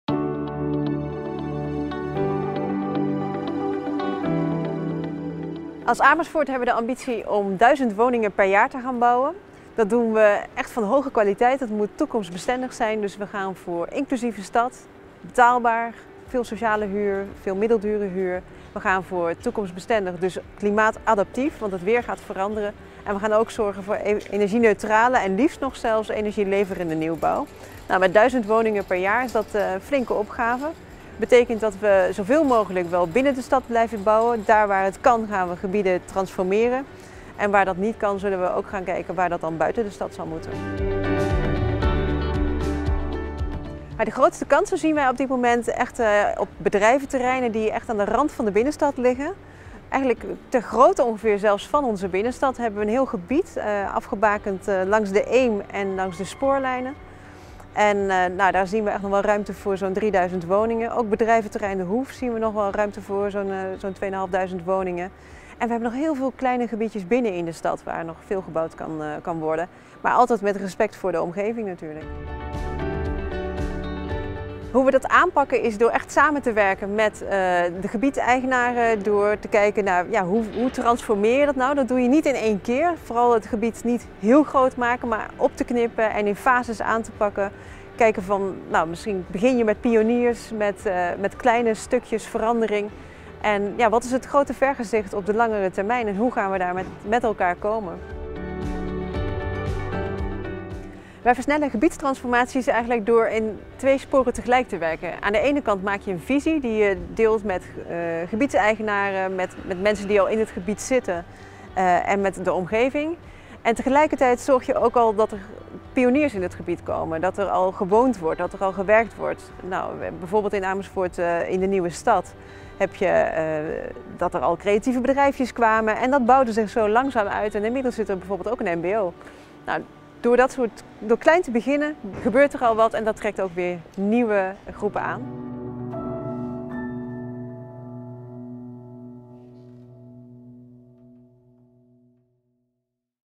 Wethouder Astrid Janssen over de transformatieopgave in Amersfoort
Ook in Amersfoort is de druk op de woningmarkt groot. Om ervoor te zorgen dat in de toekomst iedereen in Amersfoort kan blijven wonen in een woning die past, stelde de gemeente het Deltaplan wonen op. Tot 2030 groeit Amersfoort met ongeveer 1000 woningen per jaar. Maar hoe zorgt de gemeente voor toekomstbestendige woningen, die energieneutraal en betaalbaar zijn? Hierover vertelt Astrid Janssen (wethouder gemeente Amersfoort).